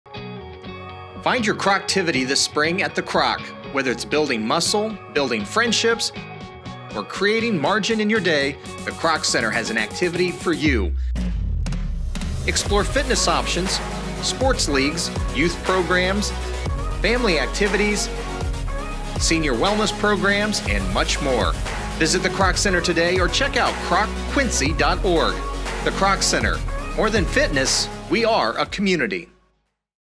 Find your Kroc-tivity! | The Kroc Center | Quincy, IL
Find your Kroc-tivity this Spring at the Quincy Kroc Center! Whether it is building muscle, building friendships, or creating margin in your day, the Kroc Center has an activity for you! Explore all the Kroc has to offer at KrocQuincy.org. More than fitness, we are a community!